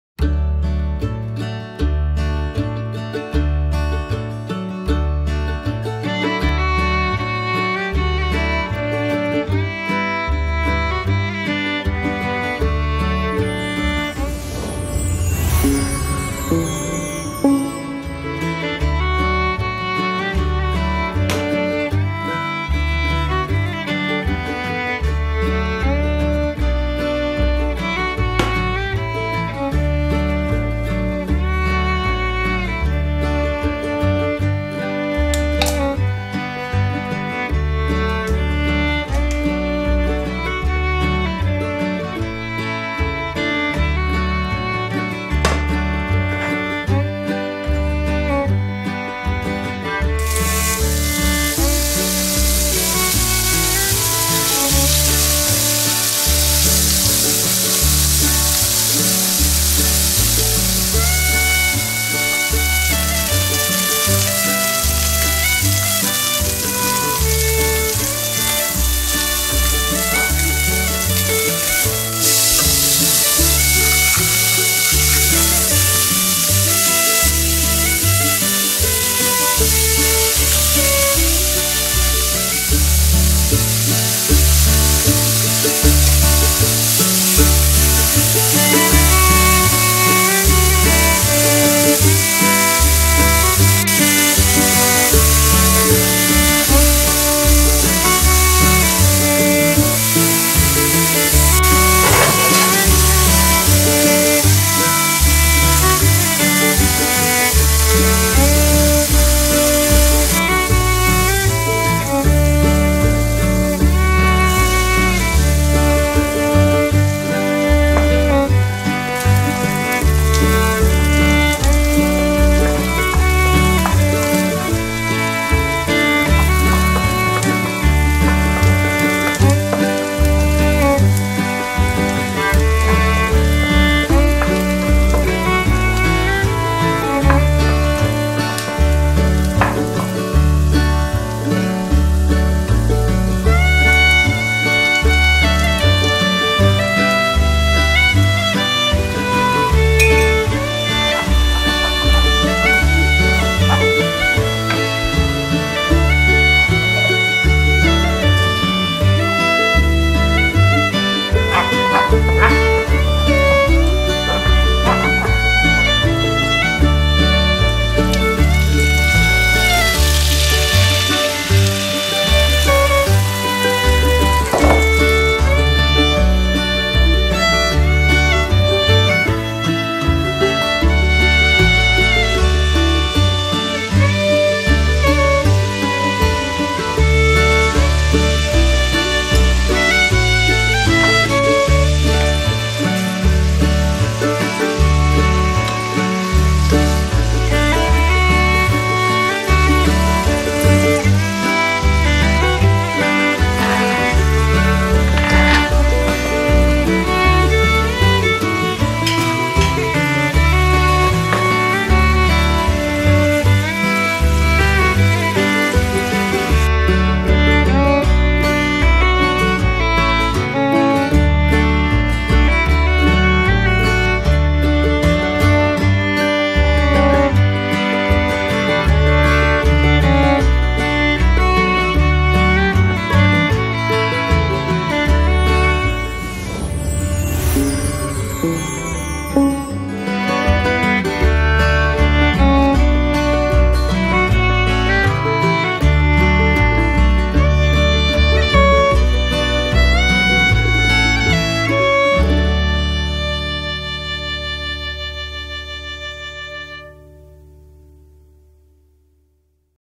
Bravčové na mede | Vnučkine recepty
Veľmi jednoduchý, rýchly a chutný recept na bravčové na mede.

V tomto videu si ukážeme ako urobiť chutný recpet z bravčového mäsa.